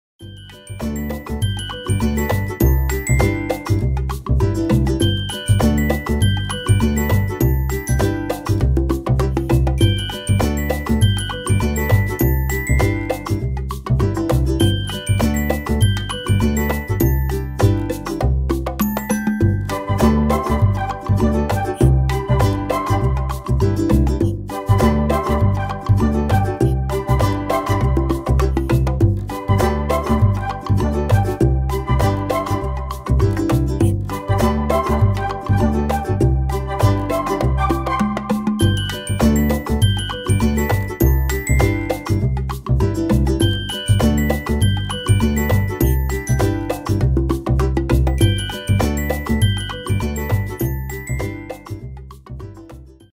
Mini Bernedoodle Boys. 5 weeks old.
The Boys! From Daisy and Ronald's litter. 5 weeks old.